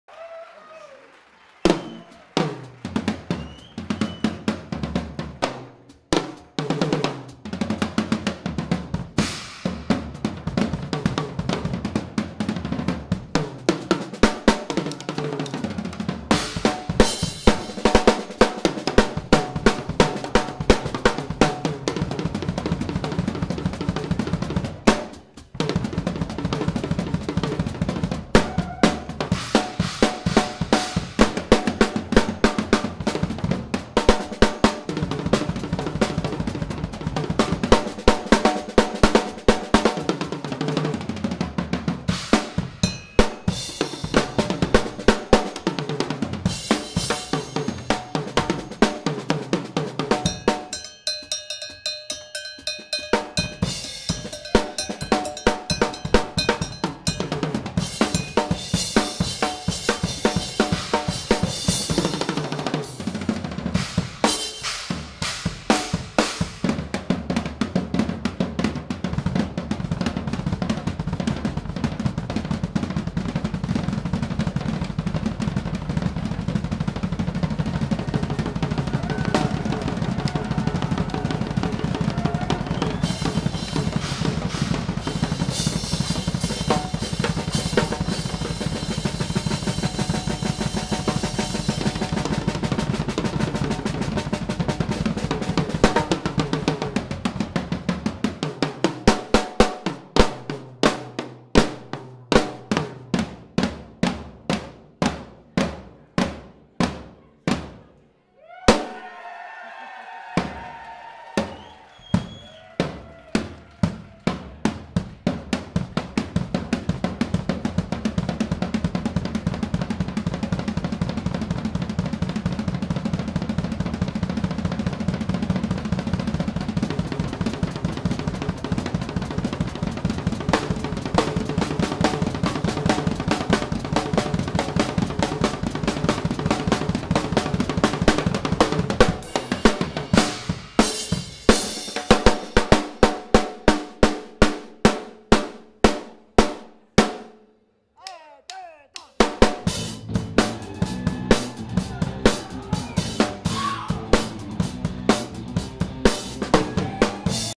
Solo de Drum David Labrecque à Sainte-Justine le 5 octobre 2019
Solo de drum David Labrecque avec Jamroad à l'école des Appalaches de Sainte-Justine pour le spectacle bénéfice Aimes-tu ma différence? Le 5 octobre 2019.